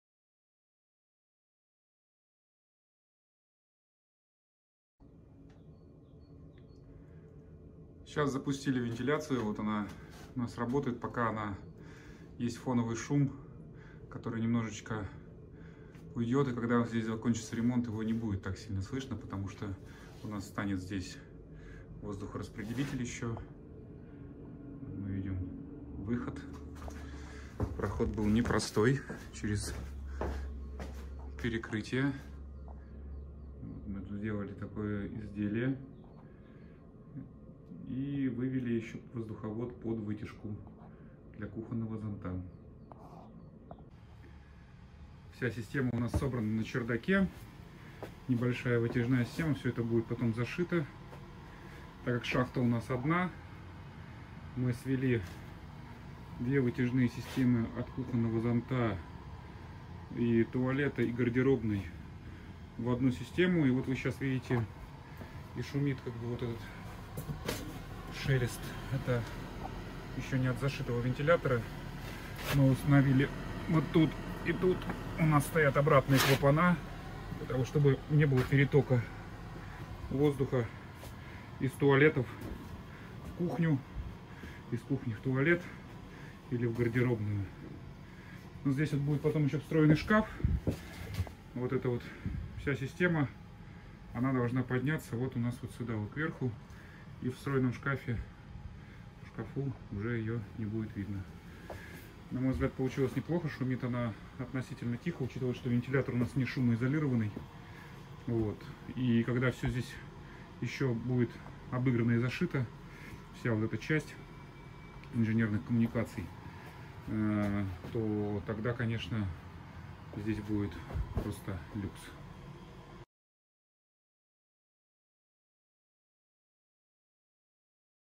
Вытяжная вентиляция в квартире старого немецкого дома
Вытяжная вентиляция в квартире, старого немецкого дома. Бесшумно и эффективно.

Вытяжная вентиляция в квартире, старого немецкого дома. Бесшумно и эффективно.    Хорошая вытяжная вентиляция конечно не решит вопрос круглогодичной вентиляции в квартире и доме, но существенно повысит комфорт в период когда температуры на улице станут приемлемыми для проветривания квартиры. Особенно такие системы актуальны в квартирах в которых не возможно реализовать сквозняк-проветривание, это квартиры окна которых выходят только на один из фасадов.
В данном примере эта система вентиляции имеет два функционала : основной это удаление запахов и влажного воздуха из ванной и кухни, а также в период когда на улице температуры от +15 до + 20 градусов при ее работе в комнатах где окна будут открыты система кондиционирования не понадобится.
В большинстве случаев пользователи не используют свои домашние вентиляционные системы в таком режиме по причине недостаточного воздухообмена и шума.
Но расчет и правильно подобранное оборудование может гарантировать тишину и производительность !!!